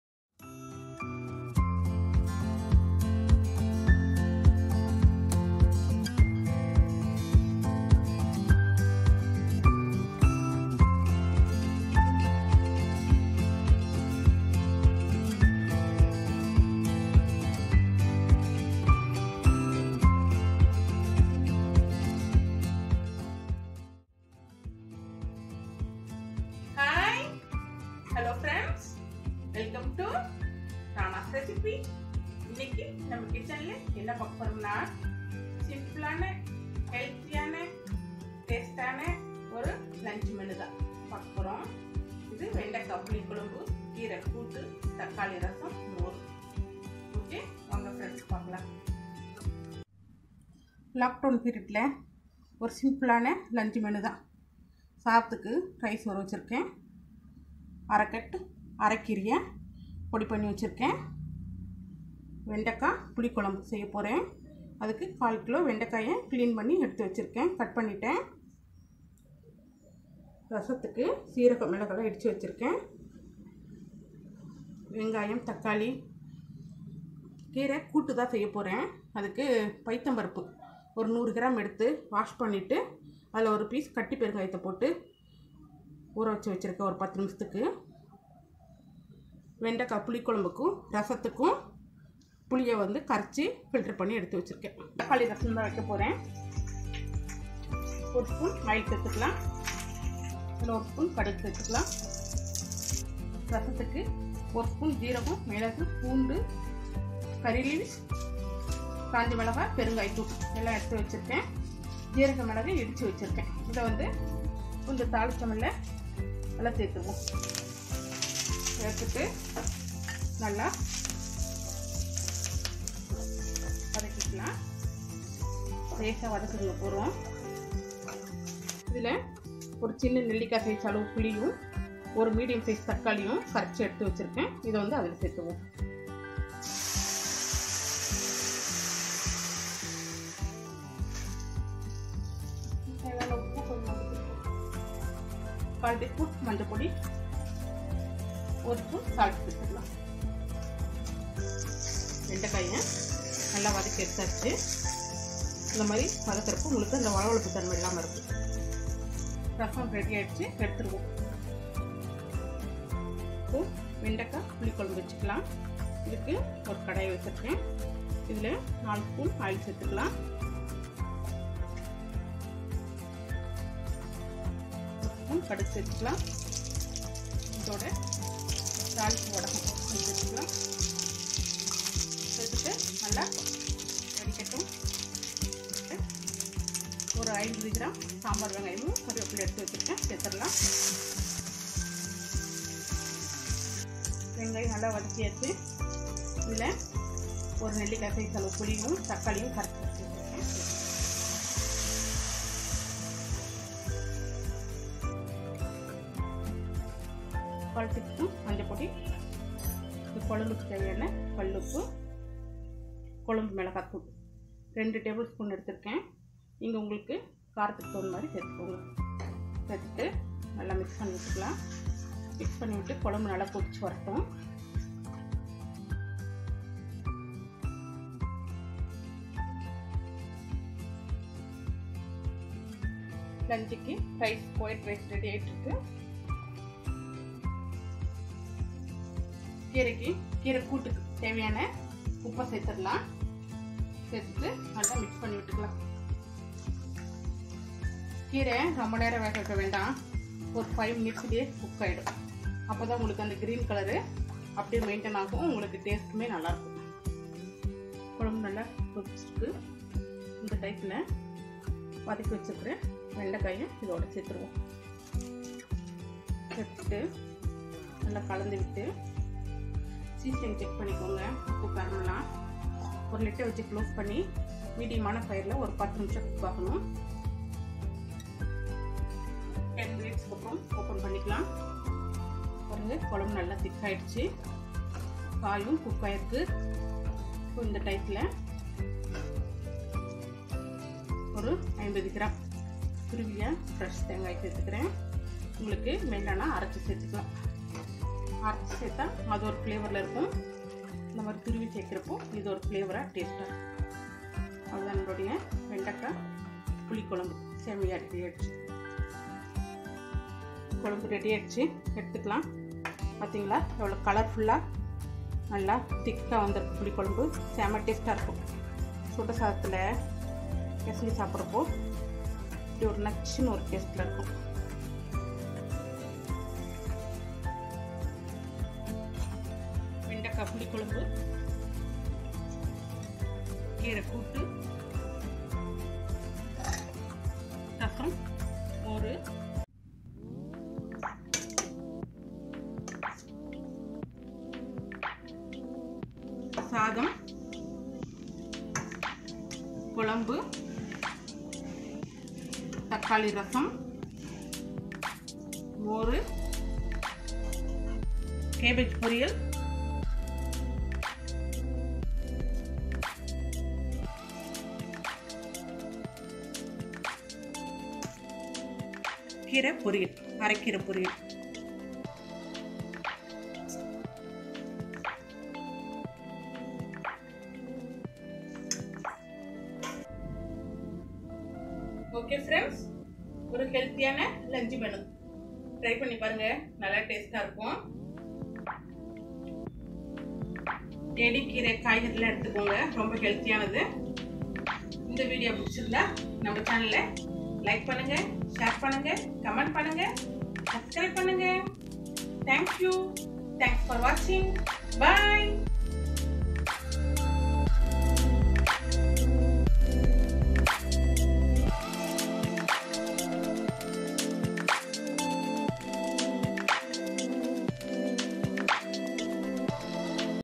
Simple Lunch Menu
00:00 Intro
00:25 Rana's Intro
00:50 Lunch Menu Ingredients
01:36 Rasam Preparation
02:33 Vendakkai Puli Kulambhu
03:53 Rice Preparation
05:50 Simple Lunch Menu Serving
06:48 Thanks Note